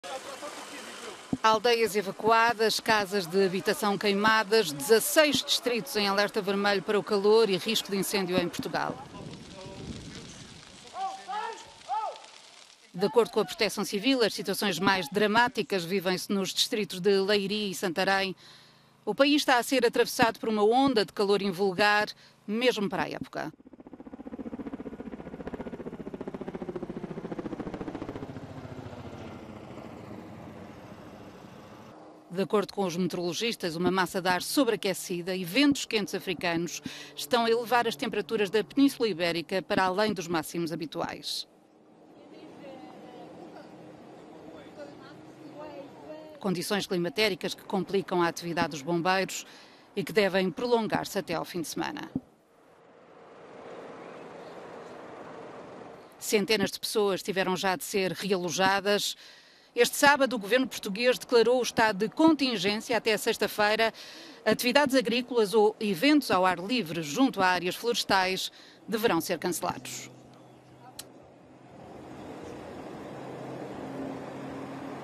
Portugal em alerta vermelho
Incêndios varrem o centro do país e já desalojaram centenas